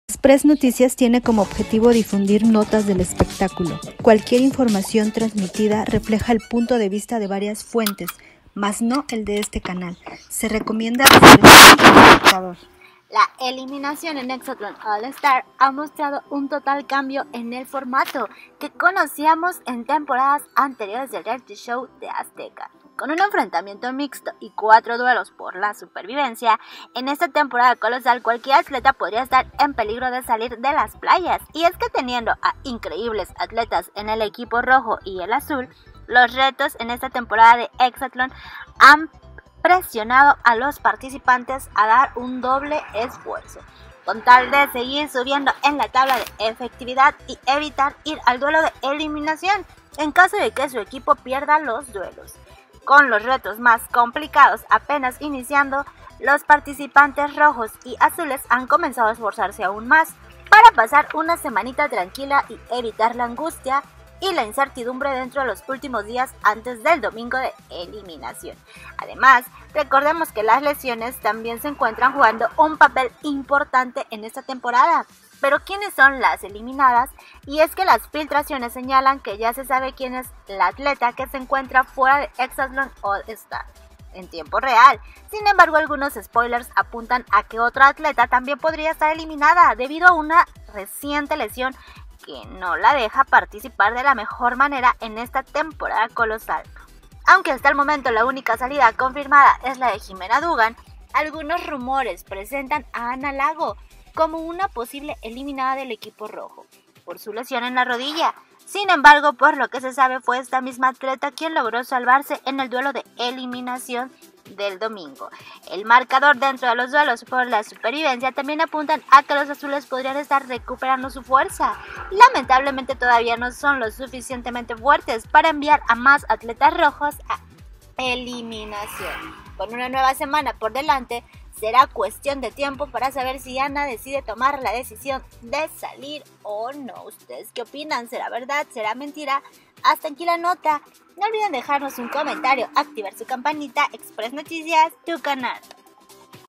Filtran eliminación DOBLE en Exatlón All Star, equipo azul y rojo pierden a un elemento
Exatlón All Star ha sido testigo del gran camino de las leyendas, lamentablemente podríamos ver a más de una despedirse esta semana.UNETE A NUESTRO CANAL HERMANO